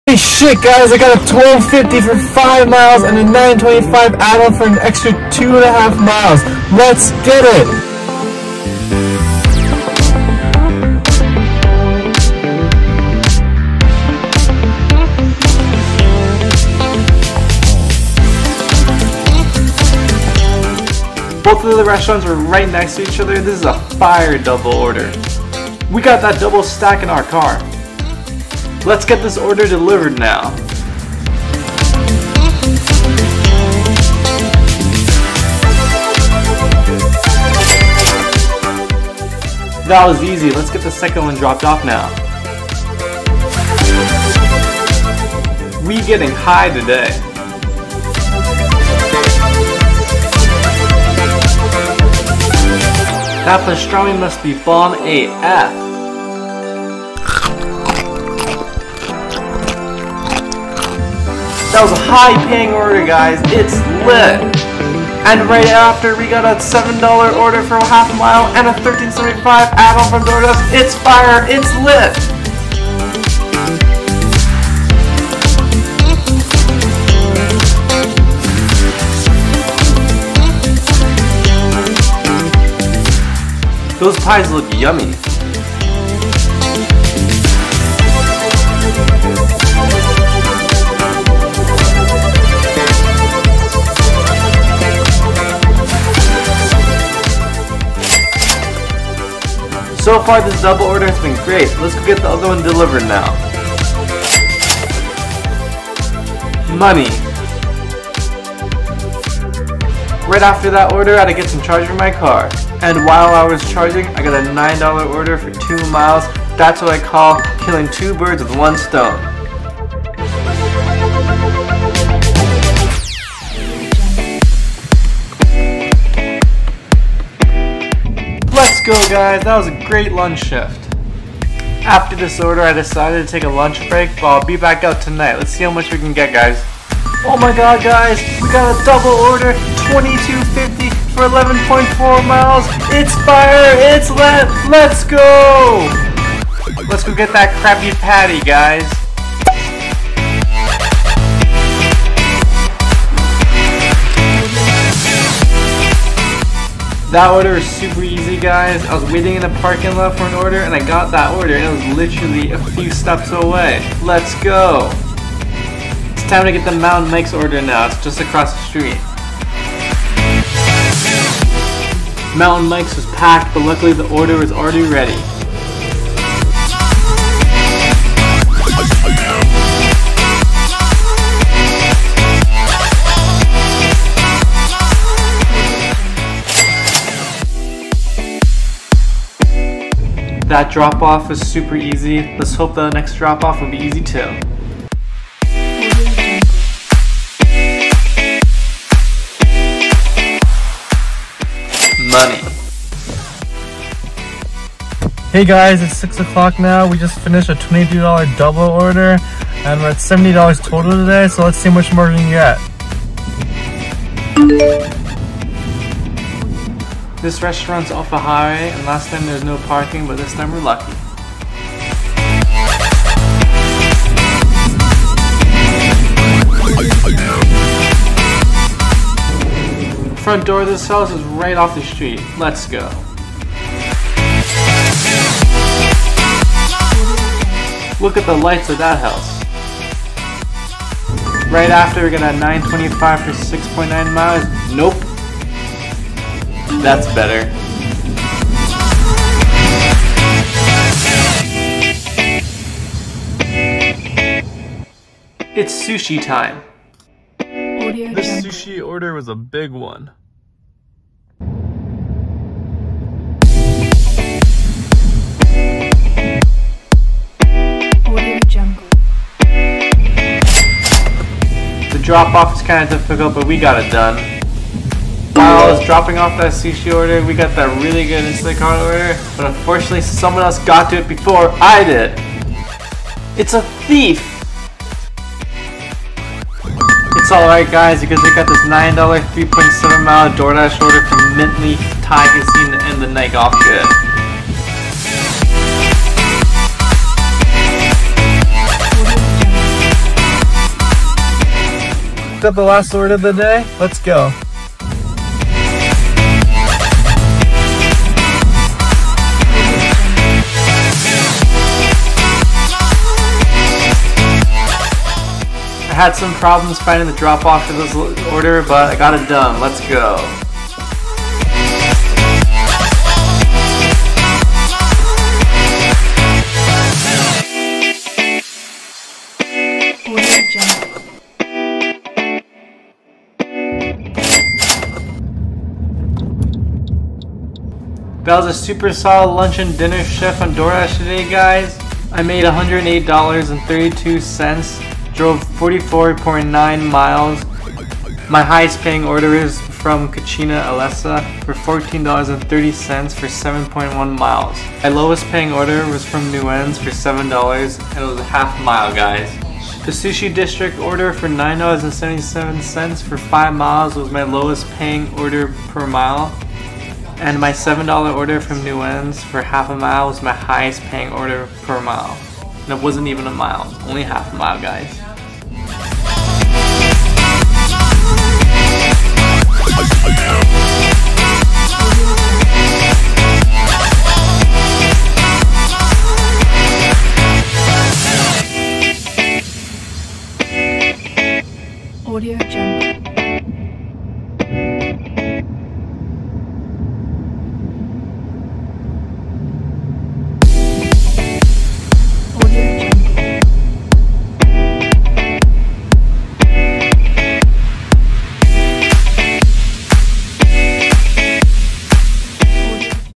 Make $100 Doordash Everyday
Follow me along as I work a friday lunch and dinner shift and make over $100 dollars!